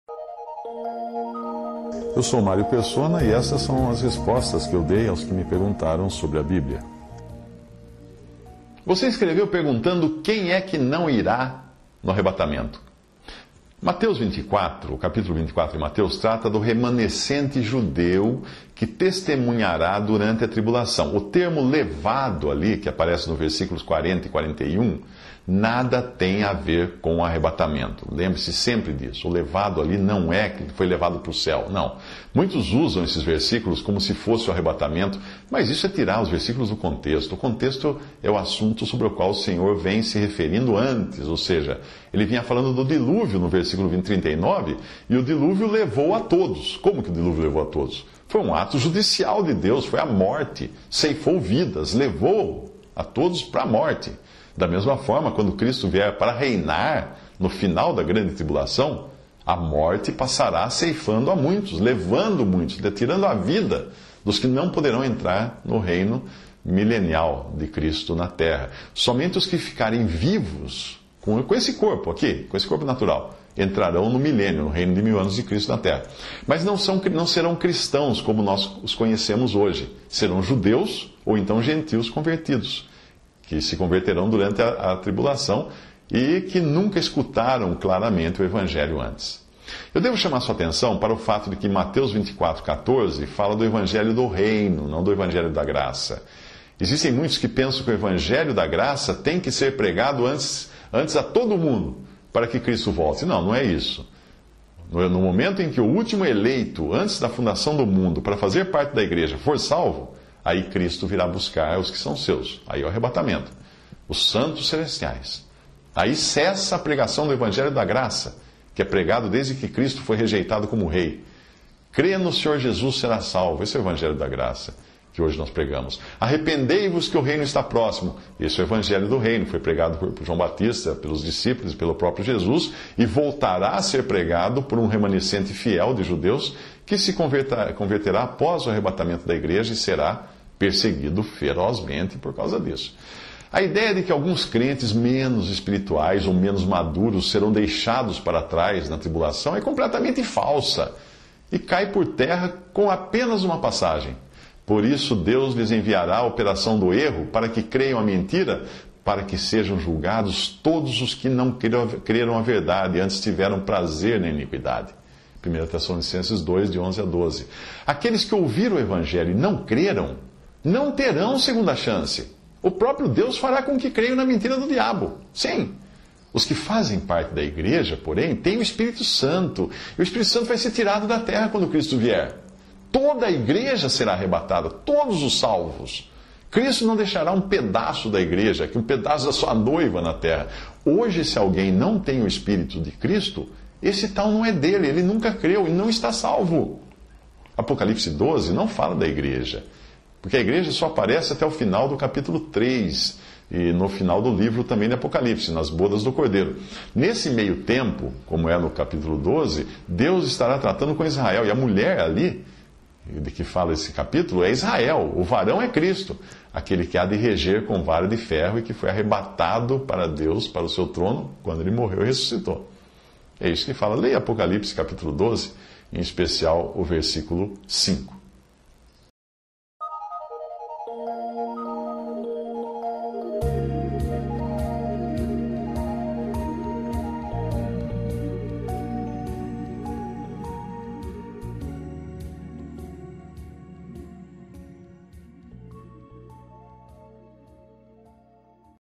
#0957 Quem não irá no arrebatamento? Mario Persona
Mateus 24 trata do remanescente judeu que testemunhará durante a tribulação. O termo "levado" que aparece nos versículos 40 e 41 nada tem a ver com o arrebatamento. Muitos usam estes versículos como sendo o arrebatamento, mas isso é tirá-los de seu contexto.